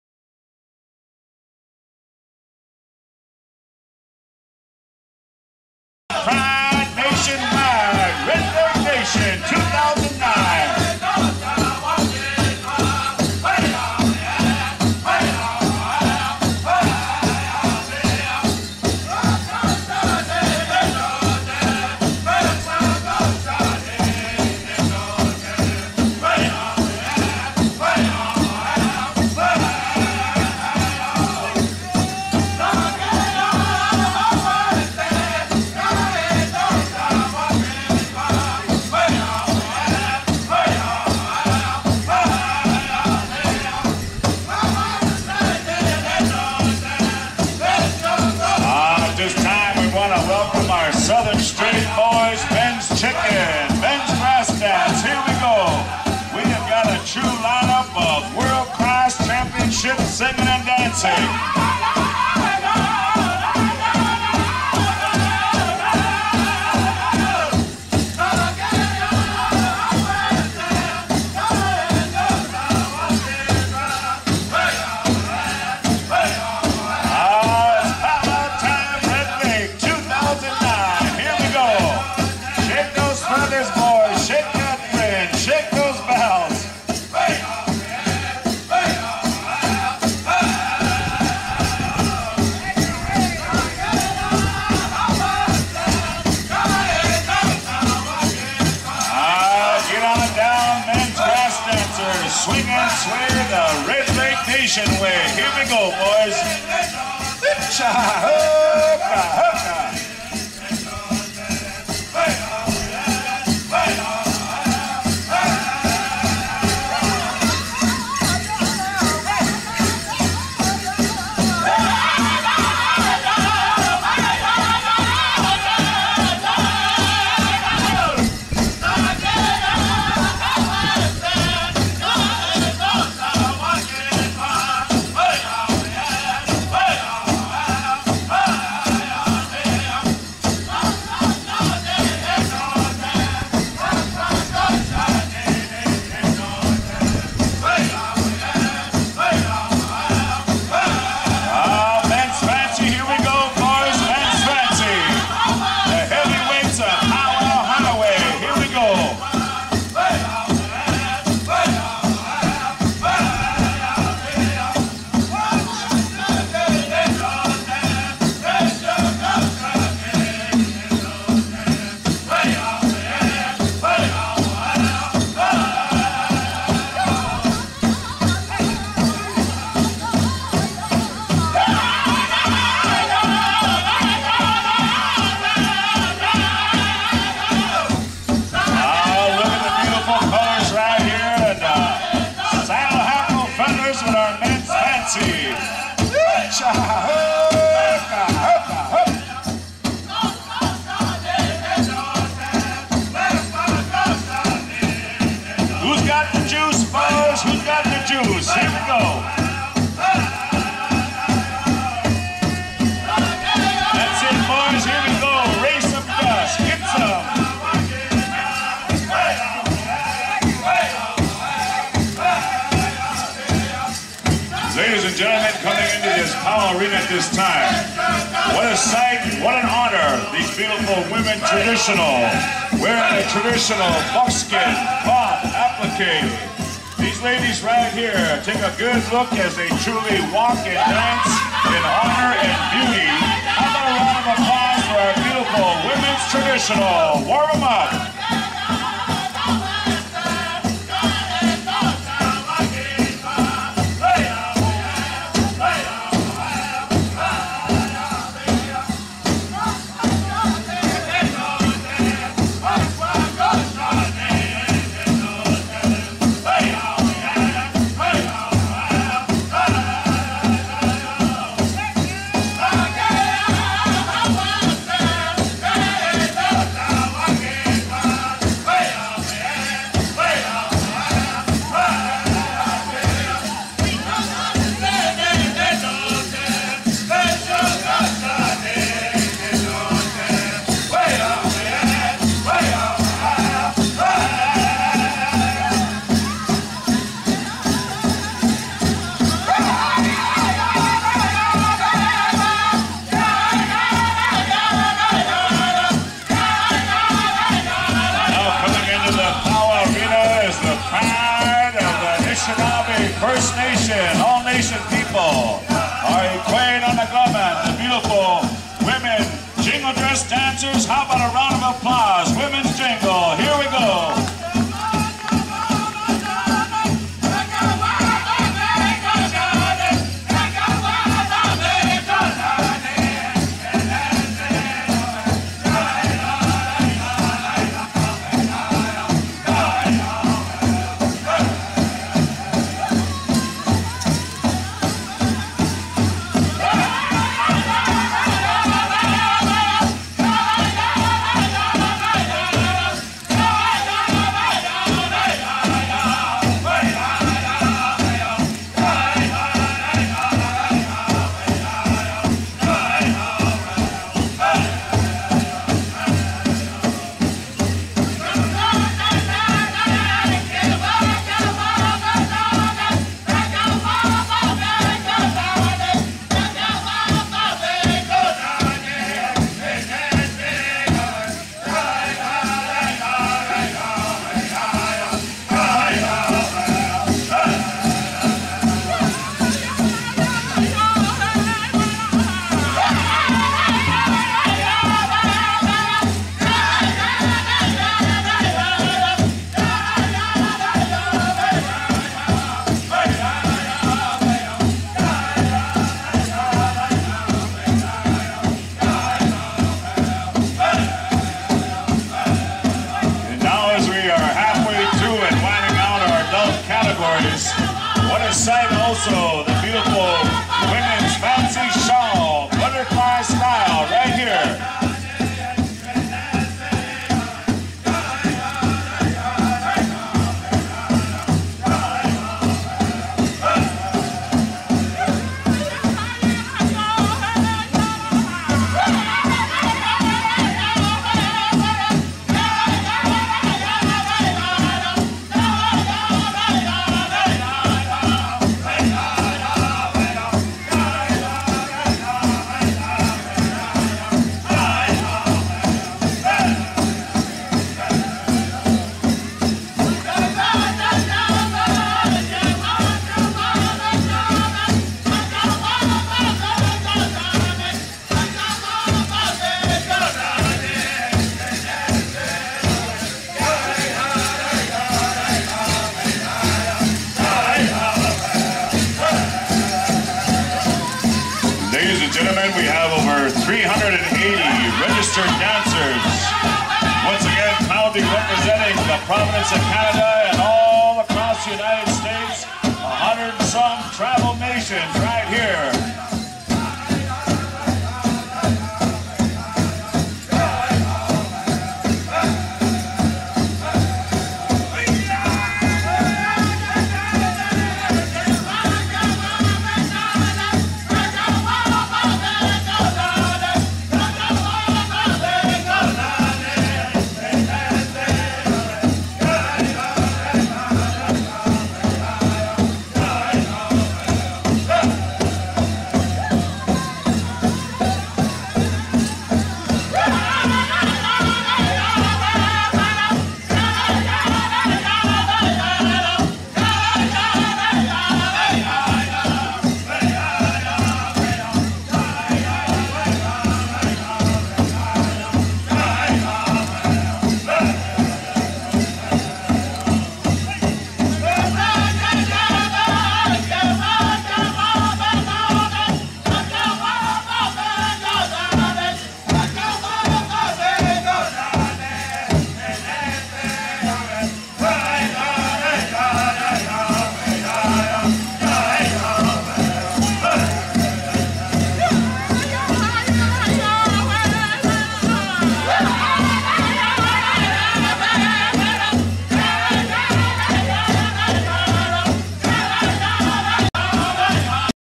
Red Lake 2009 July Celebration XVIII
Red Lake Indian Reservation annual July Celebration which took place July 6-9, 2009.